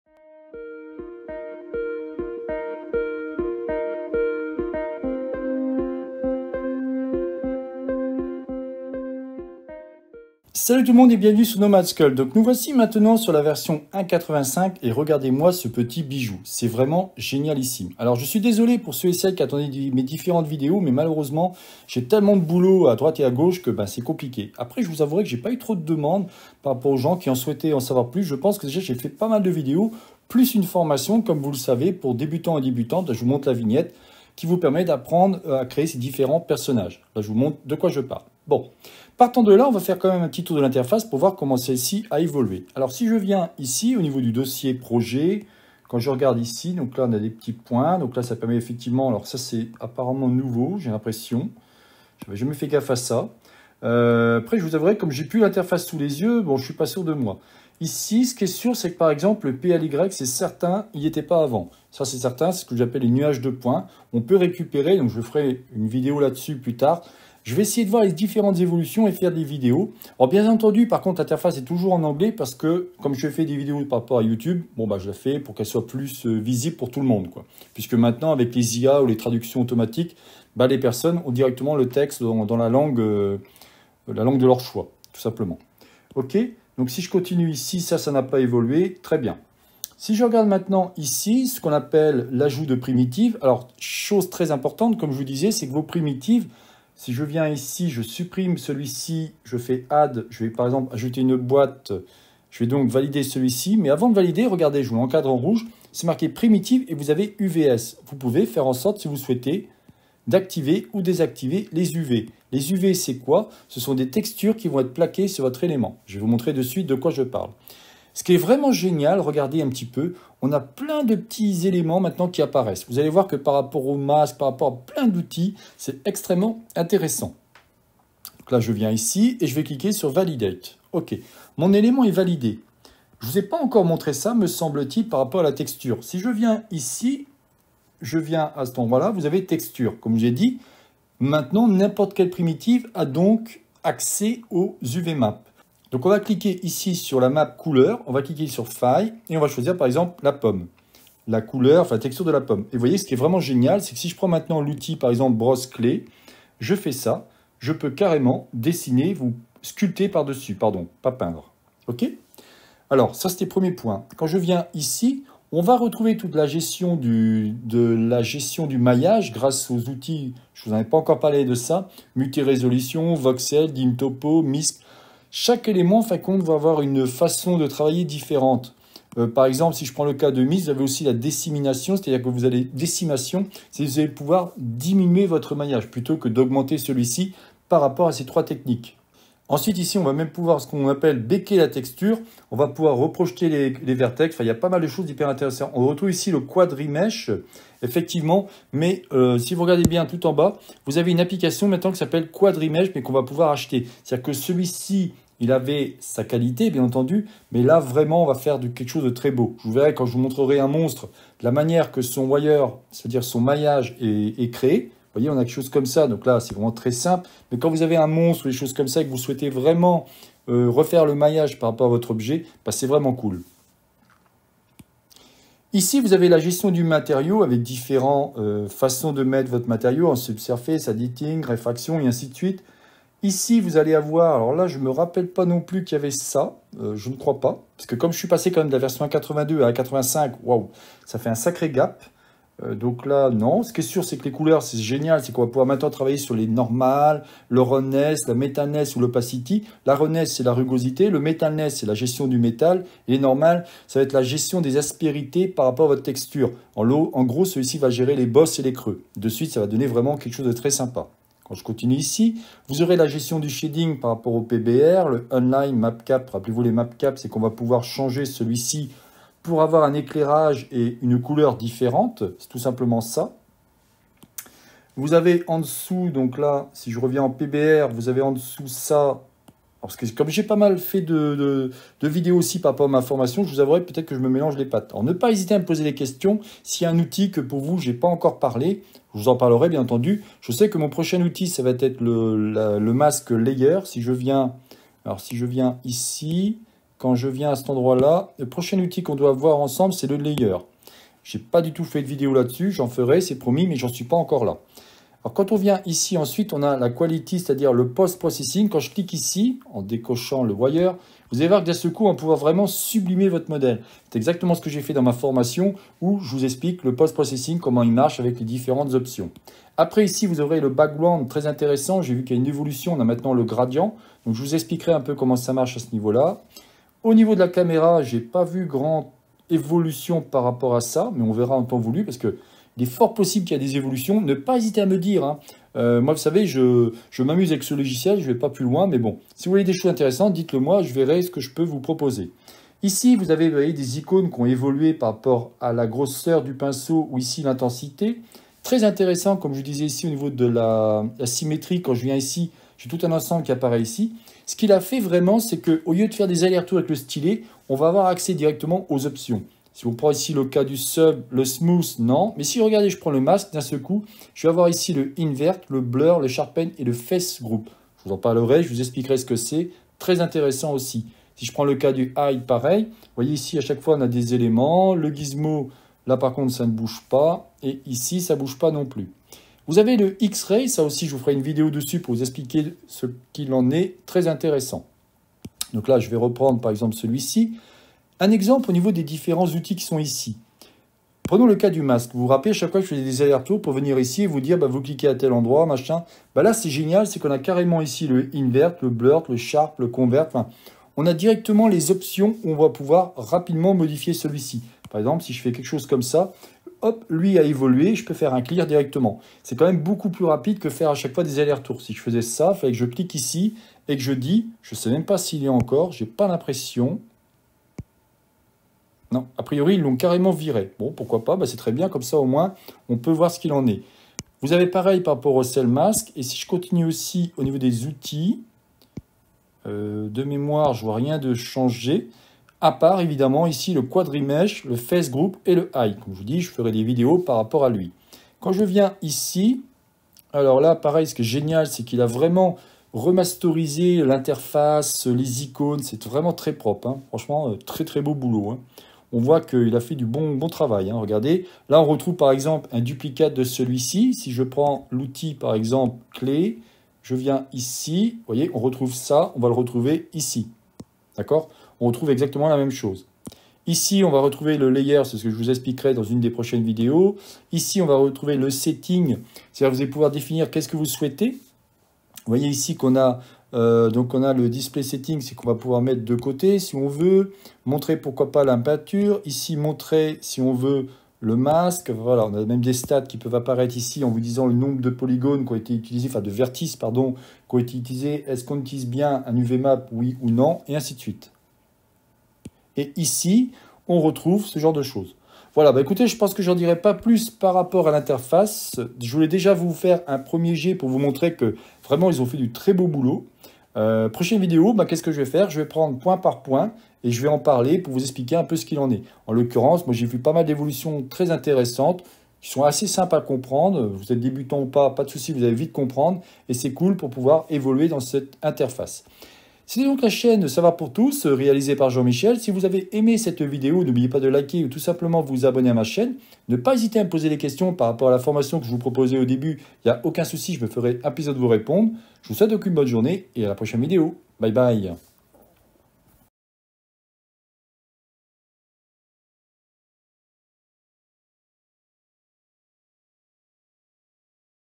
Nomad Sculpt - Evolution Interface 1.85
Allez on va commencer en premier par faire un aperçu des nouveautés sur Nomad version 1.85, puis ensuite je ferais une vidéo pour chaque outil ou fonction intéressante,